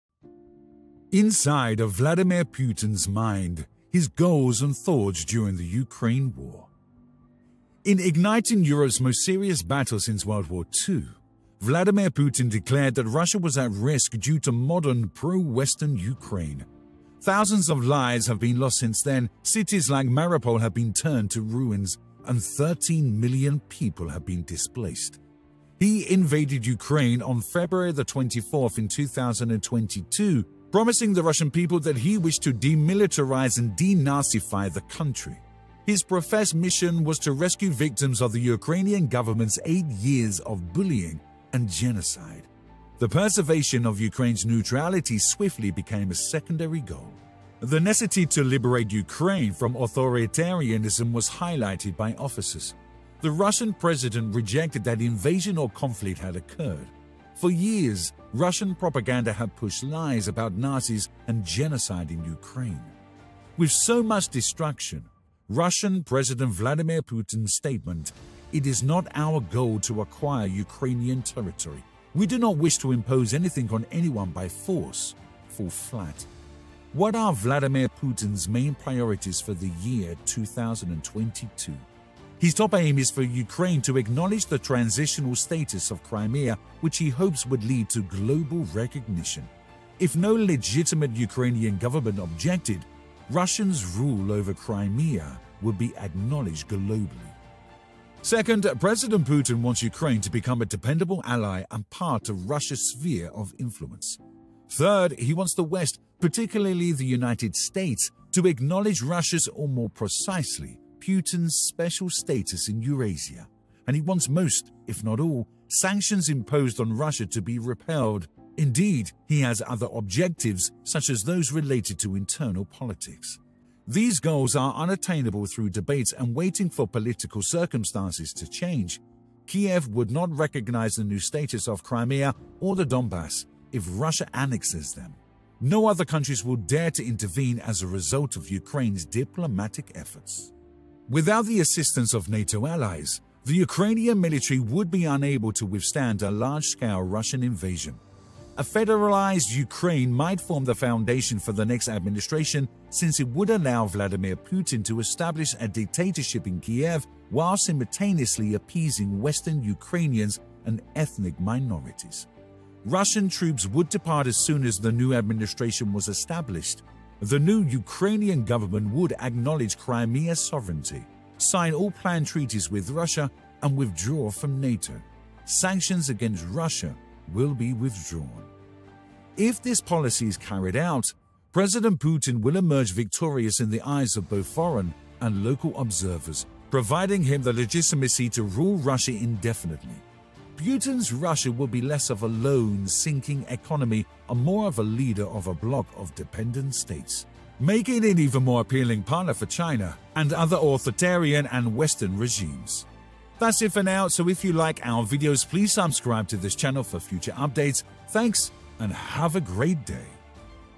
Inside of Vladimir Putin’s mind – goals during the Ukraine war
In igniting Europe's most serious battle since World War II, Vladimir Putin declared that Russia was at risk due to modern, pro-Western Ukraine.
Thousands of lives have been lost since then, cities like Mariupol have been turned to ruins, and 13 million people have been displaced.

He invaded Ukraine on February 24 in 2022, promising the Russian people that he wished to "demilitarize and de-Nazify" the country. His professed mission was to rescue victims of the Ukrainian government's eight years of "bullying and genocide." The preservation of Ukraine's neutrality swiftly became a secondary goal.

The necessity to liberate Ukraine from authoritarianism was highlighted by officers.
The Russian president rejected that invasion or conflict had occurred. For years, Russian propaganda has pushed lies about Nazis and genocide in Ukraine.

With so much destruction, Russian President Vladimir Putin's statements, "It is not our goal to acquire Ukrainian territory; we do not wish to impose anything on anyone by force," fall flat.

What are Vladimir Putin's main priorities for the year 2022?